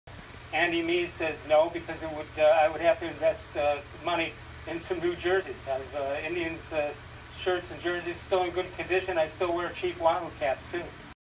Is It Time To Rename The Indians?
More Sports & Les Levine on Cleveland.com 7/6/20